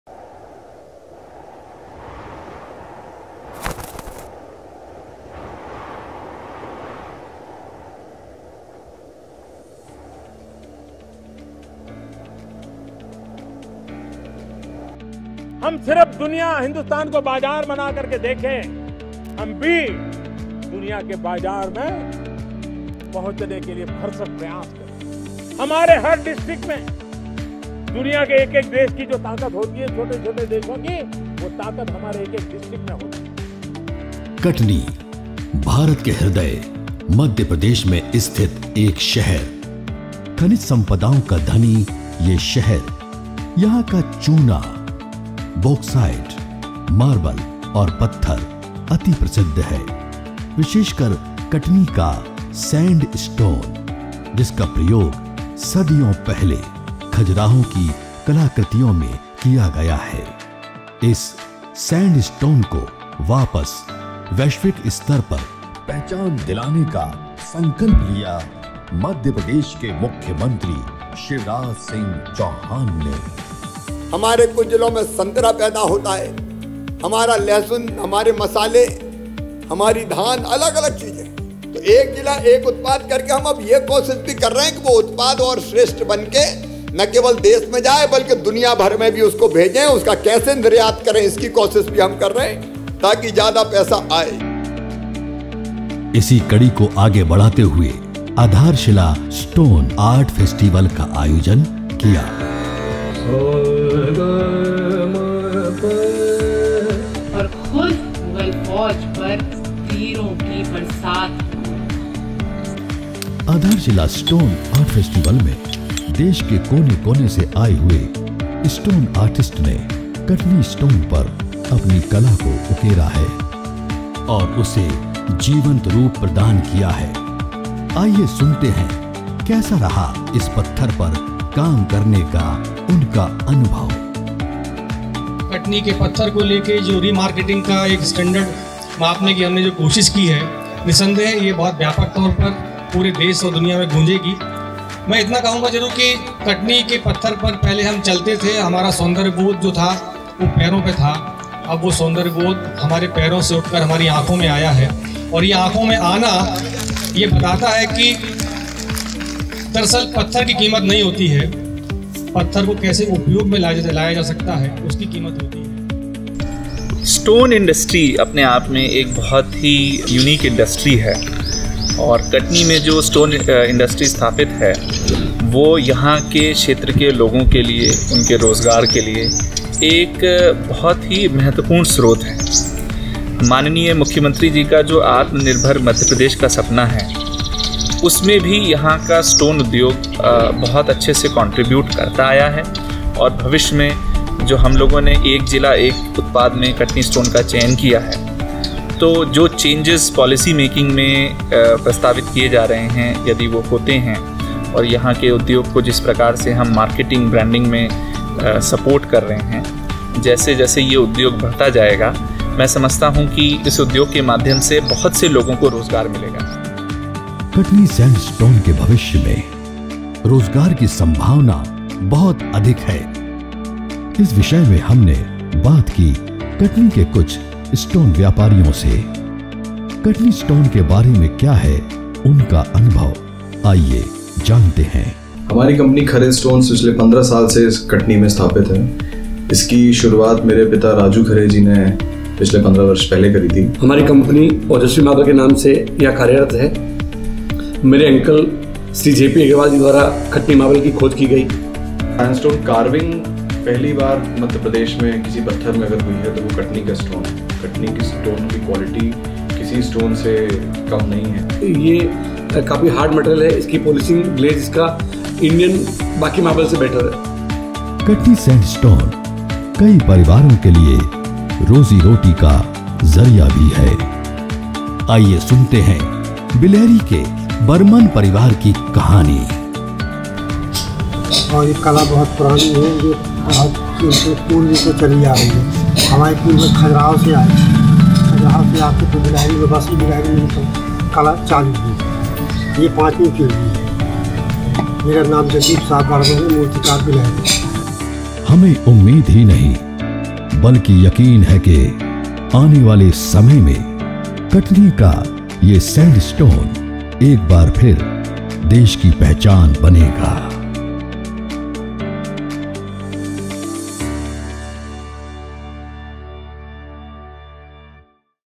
ADHARSHILA – Katni Stone Art Festival | District Katni | Madhya Pradesh Tourism
Adharshila is an amazing place where you get the opportunity to meet renowned artists and get a better understanding of how their art revolutionises cultural and social reforms. These mesmerising sculptures are made from marbles, metal scraps, and stones. Every sculpture has its own story and depicts the advanced thinking of the curator.

Katni district is located in the Northeastern part of Madhya Pradesh that forms the northern district of Jabalpur commissionerate division. It is a conglomeration of cultures from three different regions - Mahakausal, Bundelkhand, and Baghelkhand.